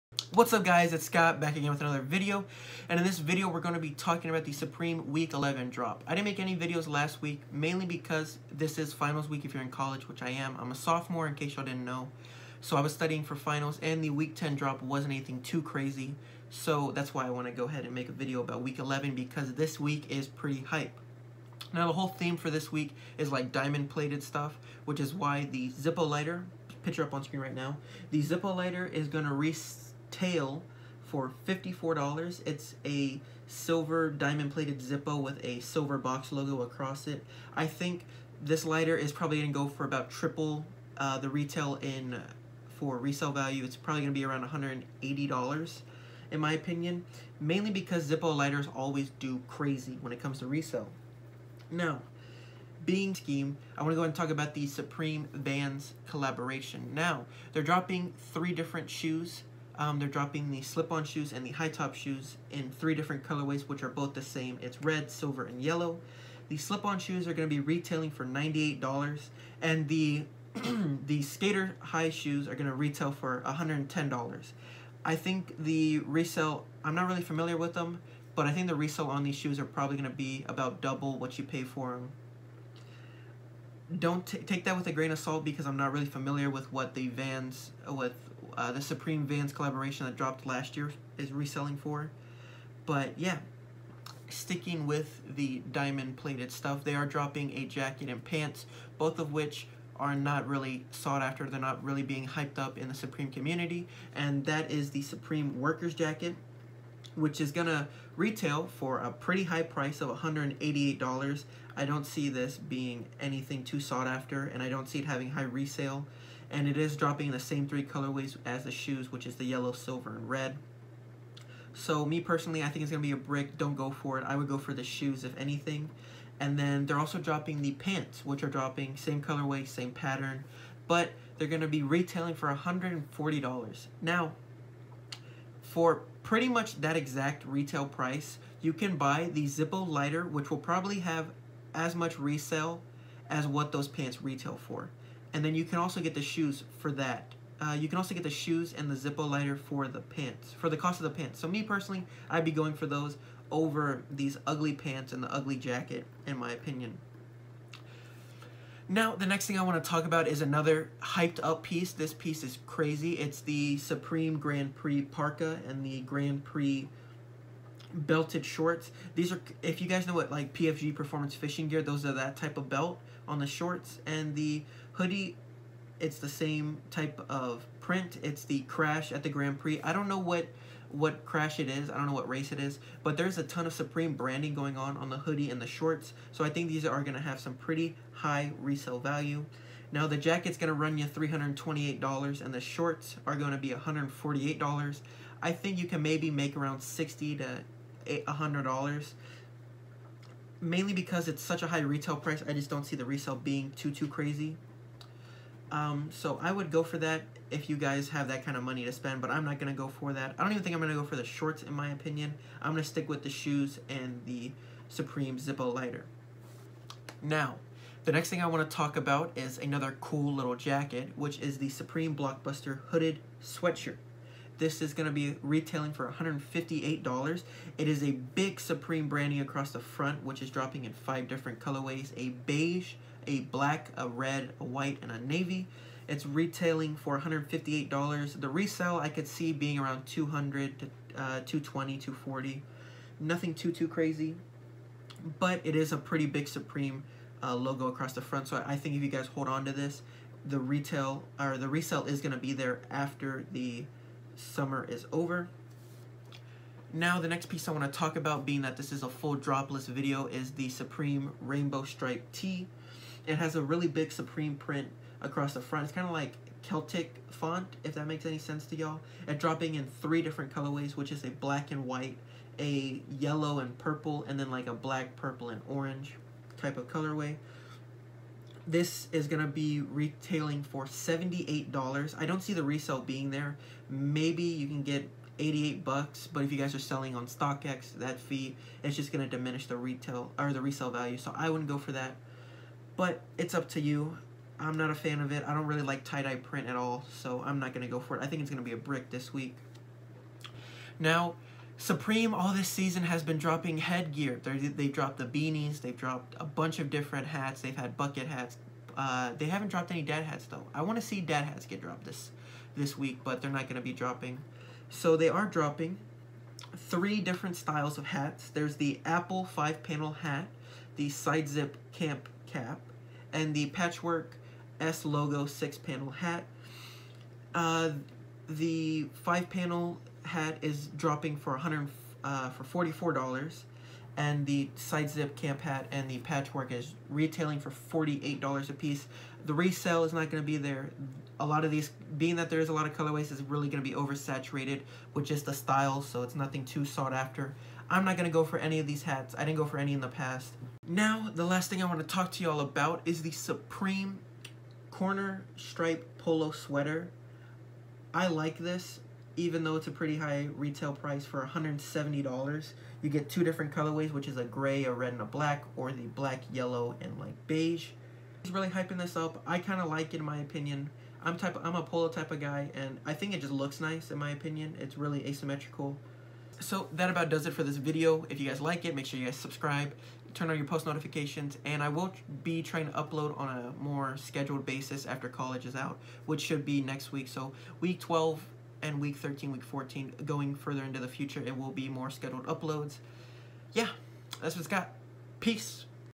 SUPREME WEEK 11 DROPLIST!
In this video I am talking about the supreme week 11